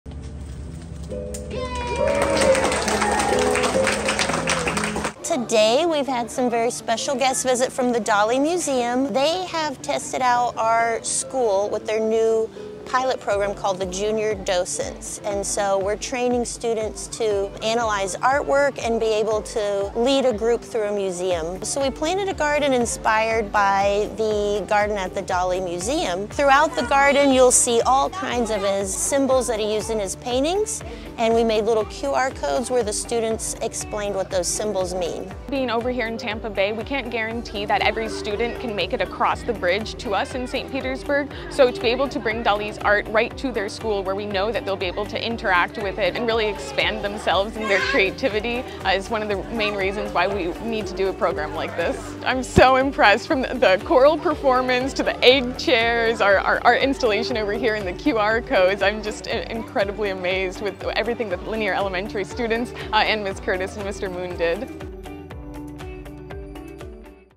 Lanier Elementary unveils Salvador Dali Avant Garden
Wednesday morning, Lanier Elementary officially unveiled its Salvador Dali Avant Garden 🌺🦋🕗

Through a partnership with The Dalí Museum, Lanier became the first school in Hillsborough County to try out the Junior Docent program on campus rather than in the museum - free of charge!